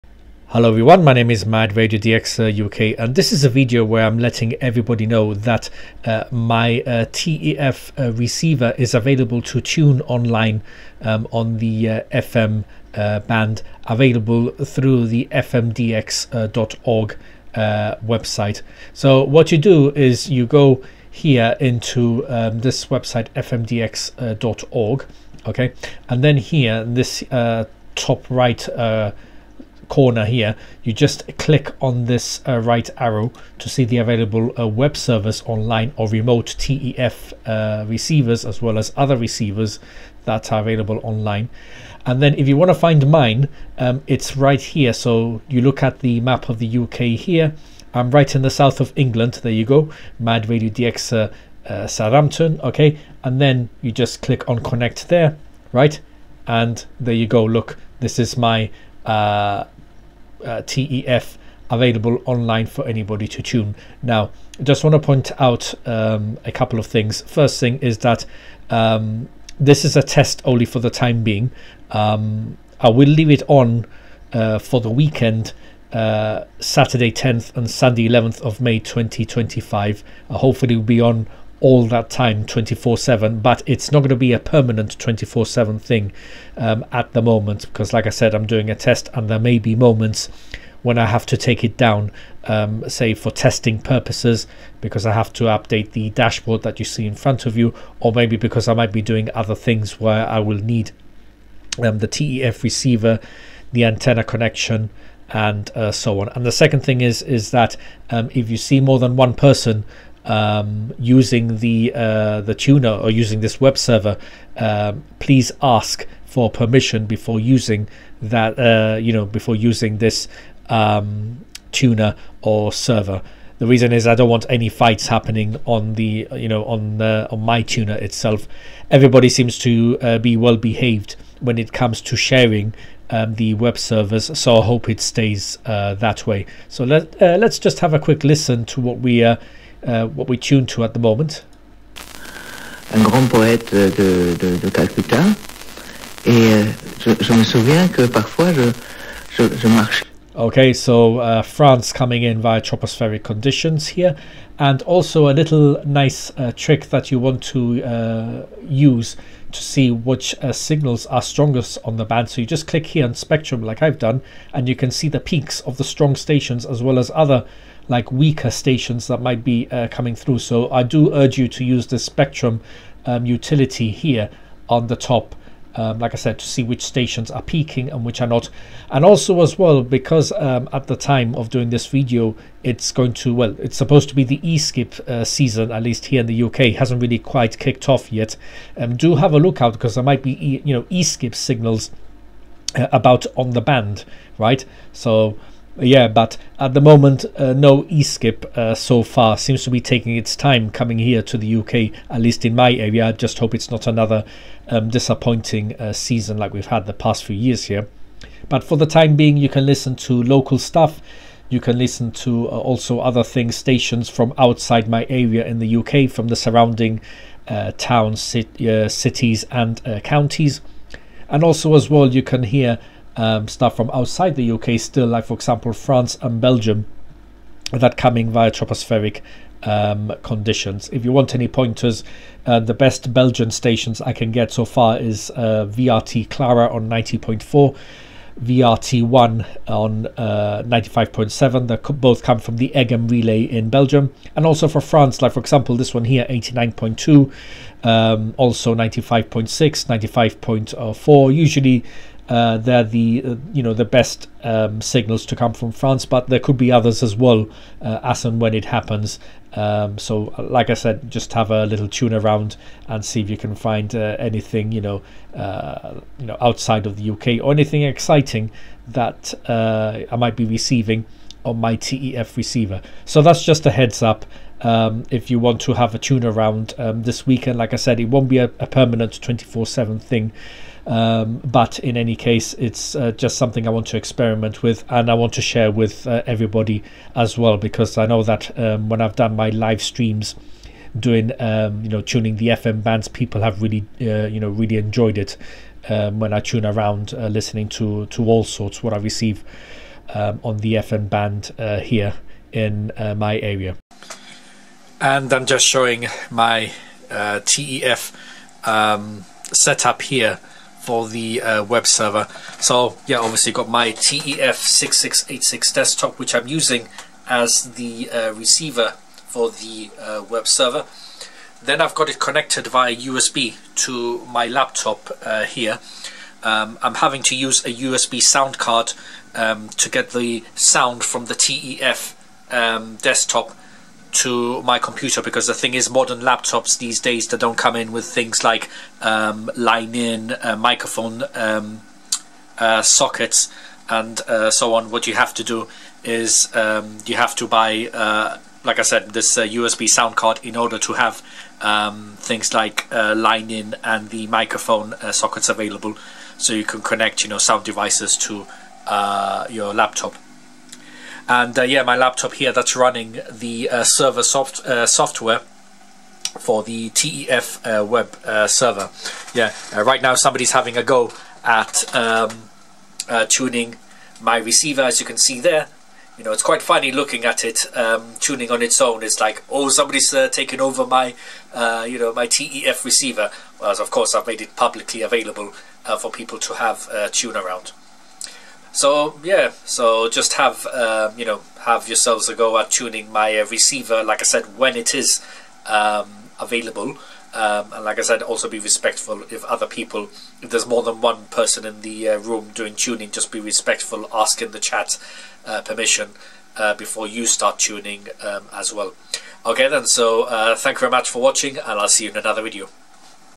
My TEF receiver is available online to tune anywhere in the world on FM band - link in description
Update (2): Thank you everyone who stopped by to have a tune of my receiver. I will be taking it offline as I have had it on almost the whole week 24/7. I will put it back on next time when conditions improve or when there are E Skip signals.
Update: Enhanced tropospheric conditions to Germany on 15th May 2025. Signals on 95.1, 96.5, 99.2, 103.3 & 106.7. Get them before they disappear. Also E Skip signals have begun to appear in my area, so keep an eye out on them.
Making an announcement that my TEF 6686 receiver is publicly available to tune online for the FM band through the FMDX.org website. Instructions on how to find my tuner are shown in the video.
Just be aware that it will not always be online in case you cannot find it, but I will try to as much as possible.

Tutorial video to install Git software, must do to clone the TEF 6686 webserver software repository...

Troubleshooting tutorial video when using Git in Command Prompt & Windows when it says "Git is not a recognised command" after installing the Git software...

00:00 Introduction & Information
00:21 How to find my receiver
01:04 Testing purposes for now
01:46 Etiquette
02:17 Quick listen to the receiver
02:35 Use the "Spectrum" feature
03:00 Look out for E Skip signals
03:28 What to listen out for
03:53 Recommended frequencies
04:33 Wanted to share for all
05:03 Showing my setup